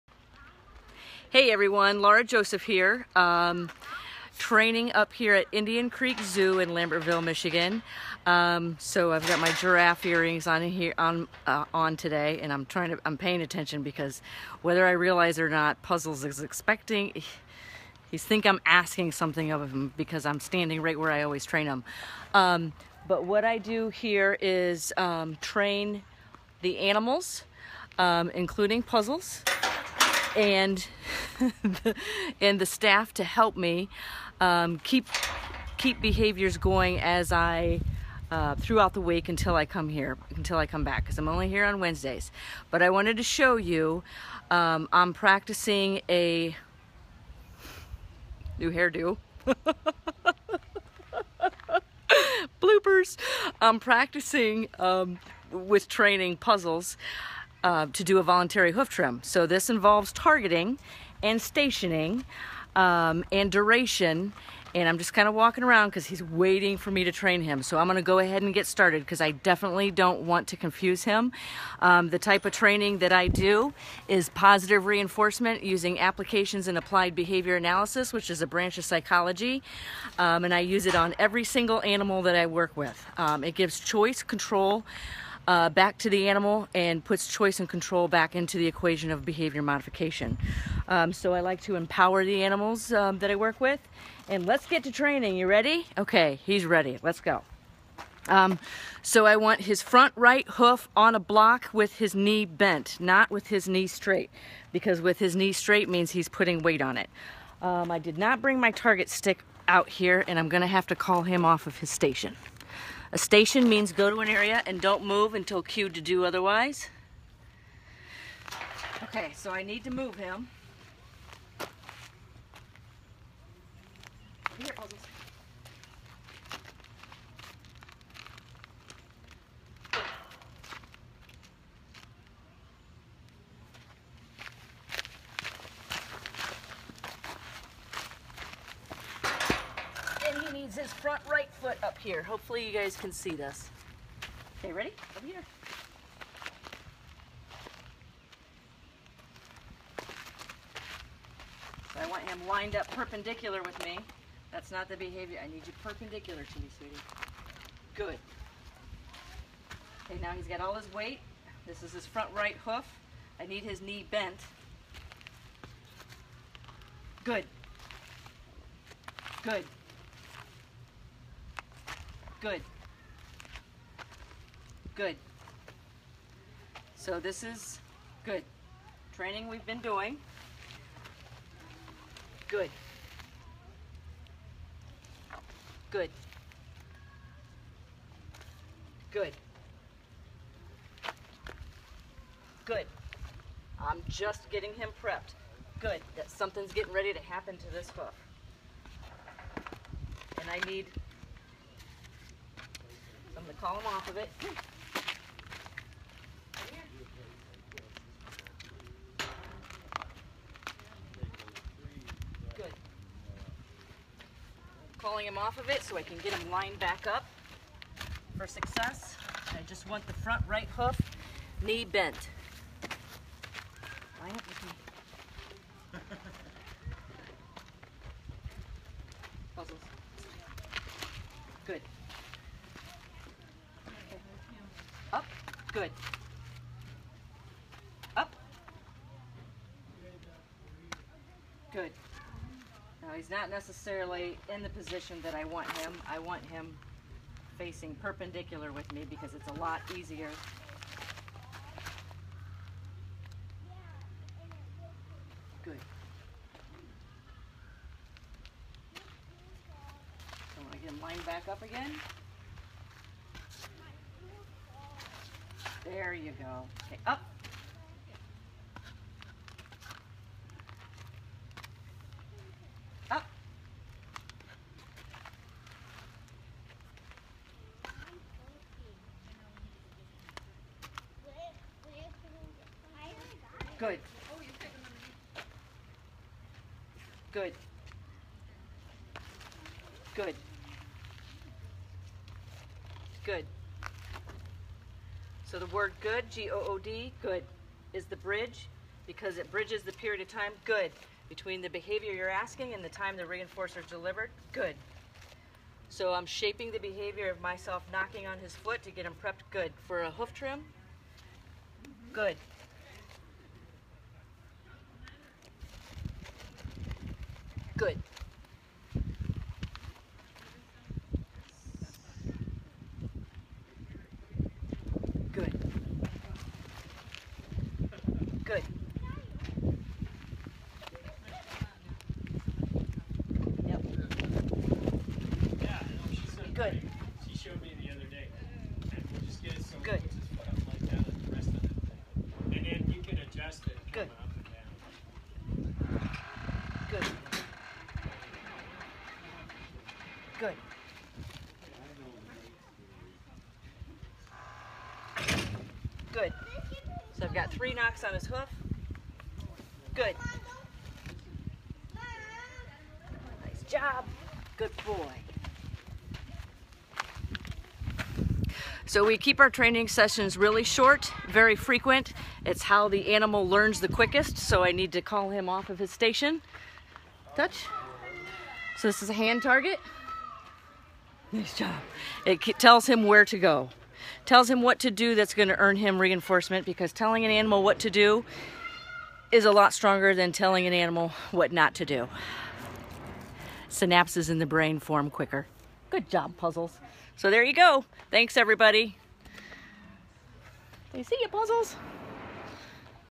giraffe hoof trimming
TheAnimalBehaviorCenter.com - a live-stream training Puzzle, the giraffe for a voluntary hoof trim. This behavior is still being shaped.